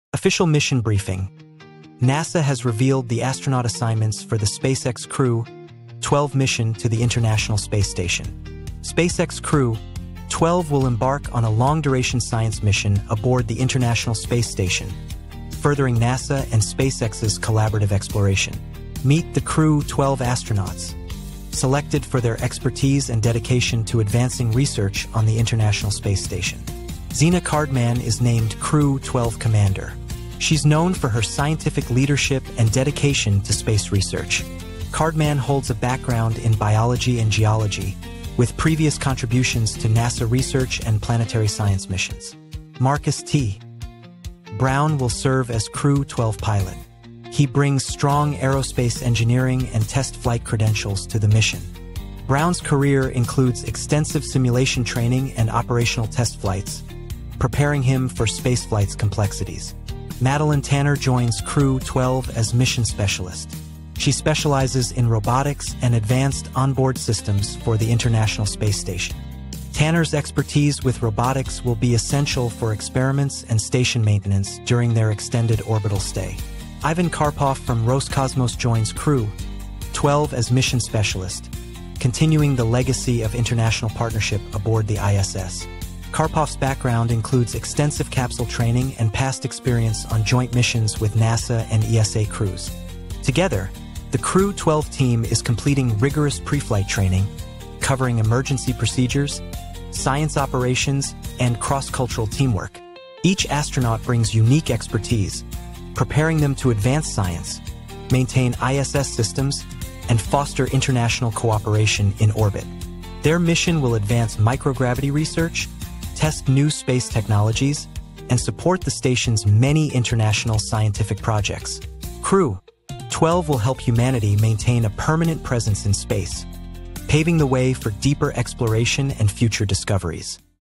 These Astronauts Are Going Where Most Will Never Go
NASA has announced the astronaut assignments for the SpaceX Crew-12 mission to the International Space Station. This marks another significant step in our ongoing "space exploration" efforts, furthering collaboration between NASA and "space x". The selected "astronauts in space" will embark on a long-duration "space mission" aboard the "international space station", advancing research and pushing the boundaries of "space travel".